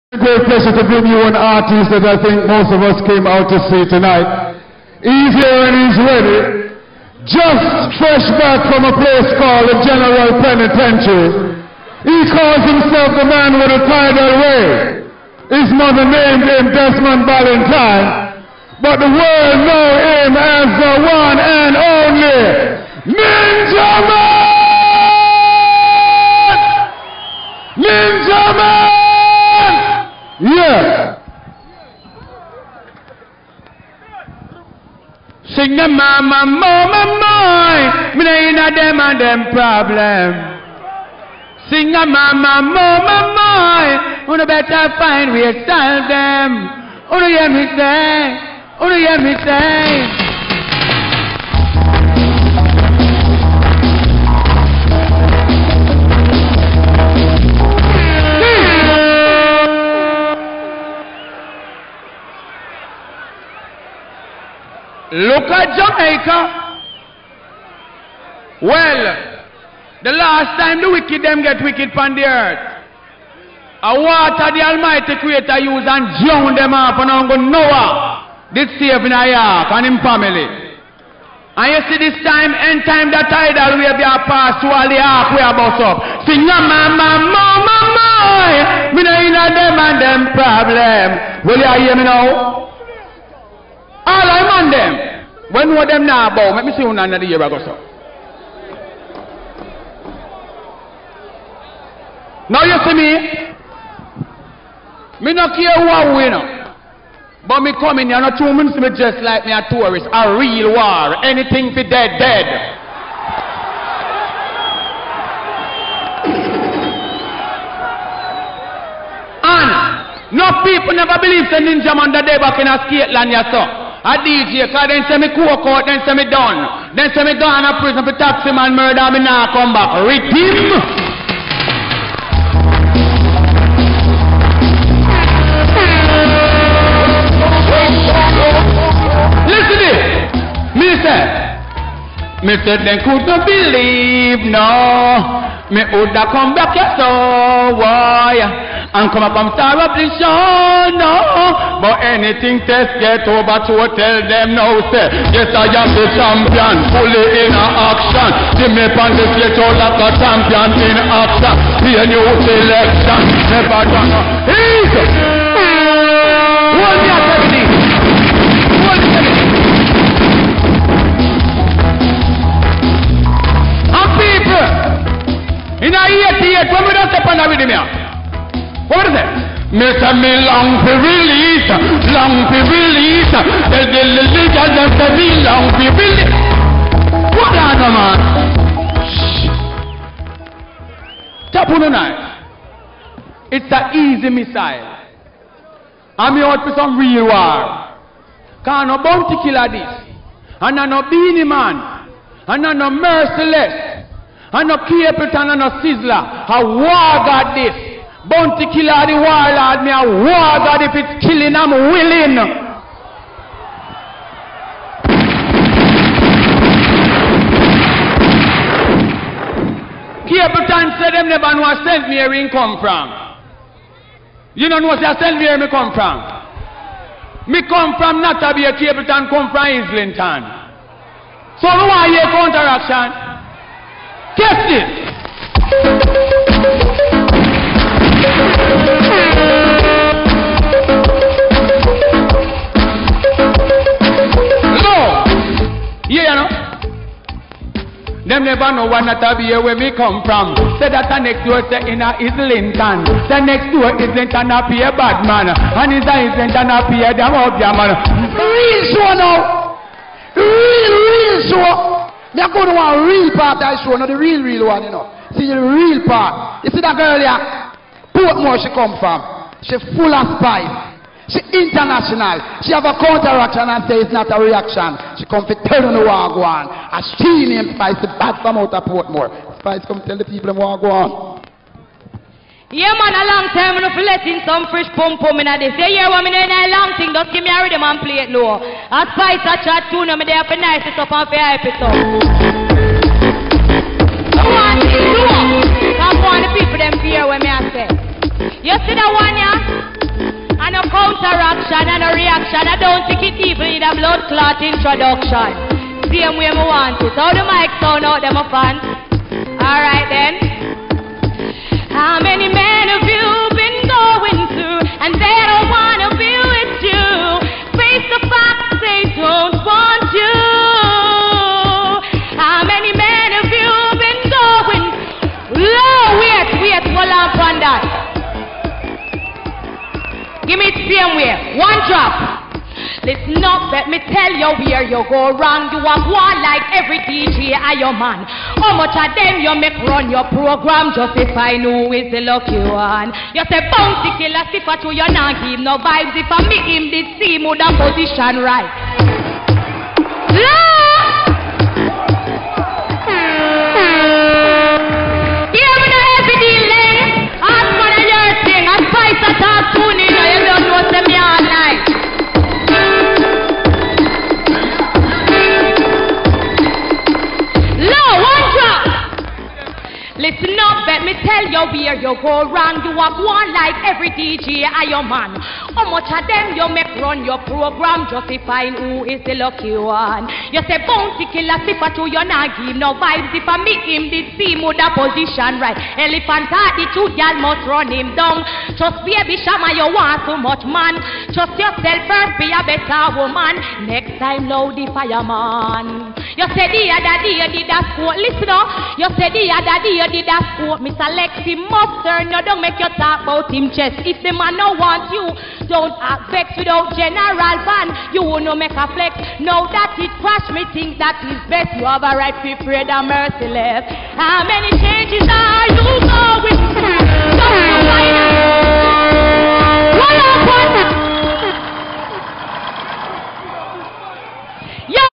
CULTURE 2000 SKATELAND,JAMAICA, WEST INDIES SPICE,NINJA MAN
THESE ARE THE DAYS OF GROWTH FOR SPICE,REGGAE/DANCEHALL ARTIST THAT HAS RISEN TO THE TOP. SPICE